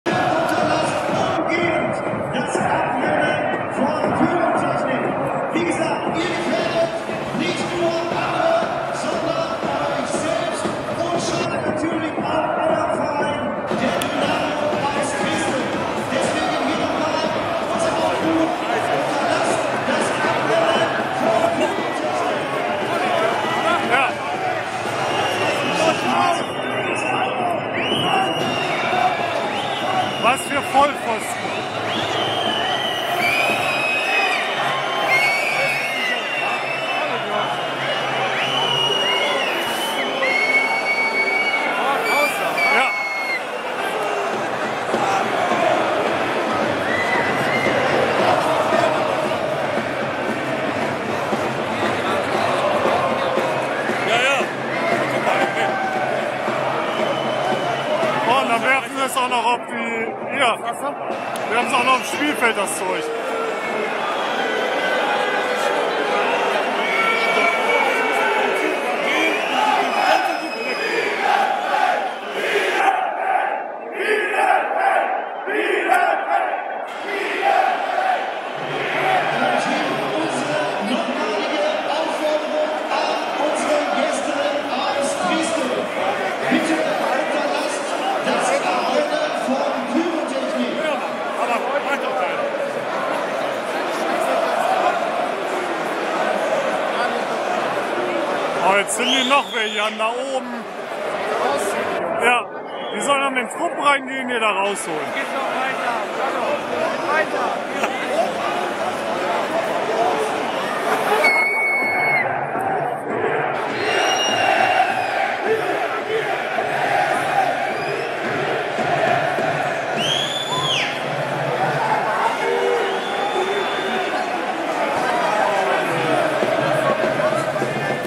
DSC vs Dynamo, Pyro von den Dynamo Fans im Block, 061213,
Fans von Dynamo zünden Pyrotechnik im Block beim Spiel DSC vs Dynamo Dresden am 06.12.13.